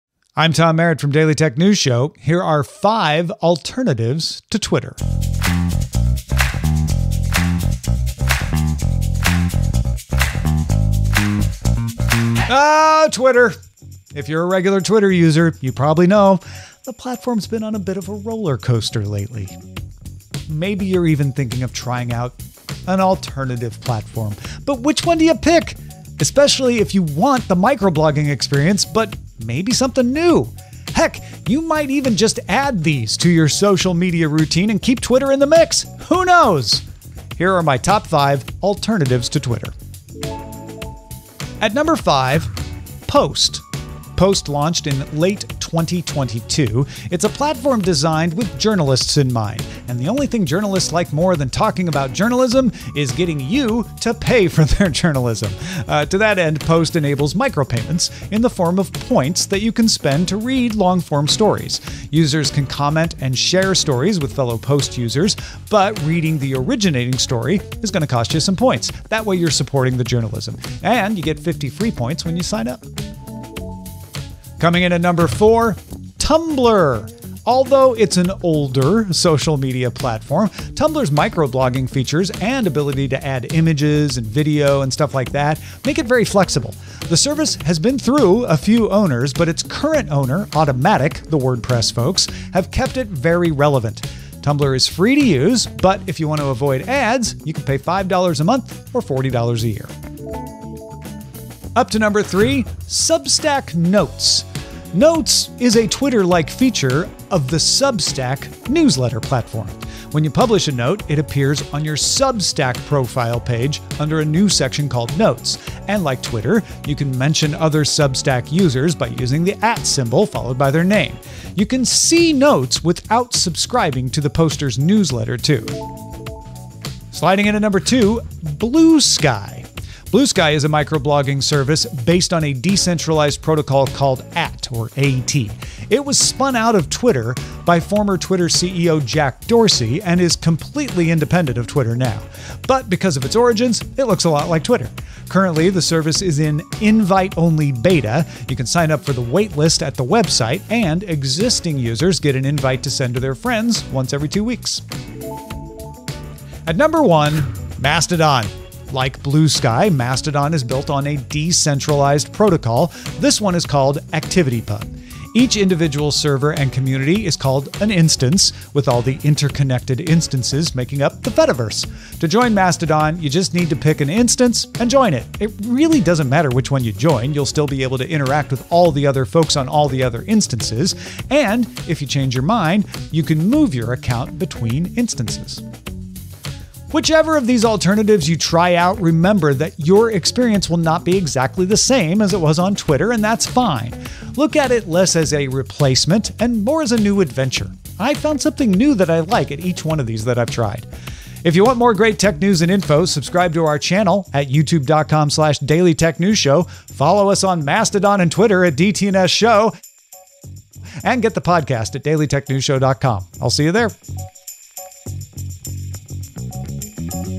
Top Twitter Alternatives - Tom's Top Five
In this video:
If you’re a regular Twitter user you probably know the platform has been on a bit of a roller coaster lately. You might even be thinking of trying out an alternative platform. But which one do you pick? Here are Tom's Top Five alternatives to Twitter!